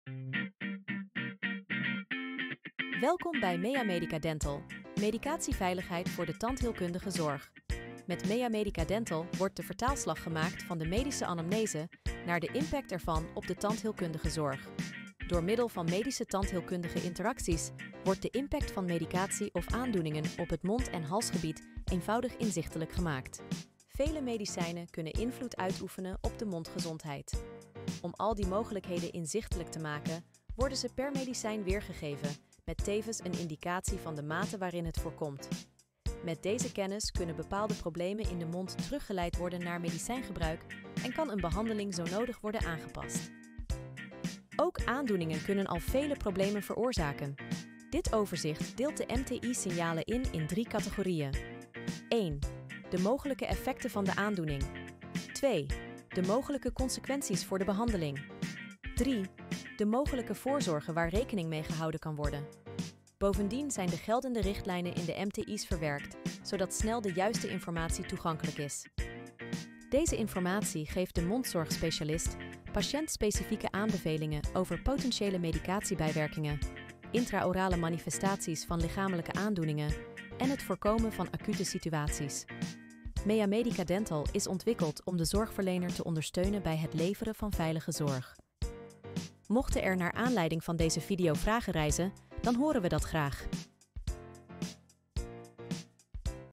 MTI signalen in meamedica | dental: medicatieveiligheid voor de tandheelkundige zorg
Met meamedica | dental wordt de vertaalslag gemaakt van de medische anamnese naar de impact ervan op de tandheelkundige zorg.
Door middel van Medisch Tandheelkundige Interacties wordt de impact van medicatie of aandoeningen op het mond – halsgebied eenvoudig inzichtelijk gemaakt.
Vele medicijnen kunnen invloed uitoefenen op de mondgezondheid. Om al die mogelijkheden inzichtelijk te maken worden ze per medicijn weergegeven met tevens een indicatie van de mate waarin het voorkomt.
Met deze kennis kunnen bepaalde problemen in de mond teruggeleid worden naar medicijngebruik en kan een behandeling zonodig worden aangepast.

Ook aandoeningen kunnen al vele problemen veroorzaken. Dit overzicht deelt de MTI signalen in in drie categorieën:
1. De mogelijke effecten van de aandoening
2. De mogelijke consequenties voor de behandeling
3. De mogelijke voorzorgen waar rekening mee gehouden kan worden
Bovendien zijn de geldende richtlijnen in de MTIs verwerkt zodat snel de juiste informatie toegankelijk is.

Deze informatie geeft de mondzorg specialist patiënt-specifieke aanbevelingen over potentiële medicatiebijwerkingen, intraorale manifestaties van lichamelijke aandoeningen en het voorkomen van acute situaties. Meamedica | dental is ontwikkelt om de mondzorgverlener te ondersteunen bij het leveren van veilige zorg.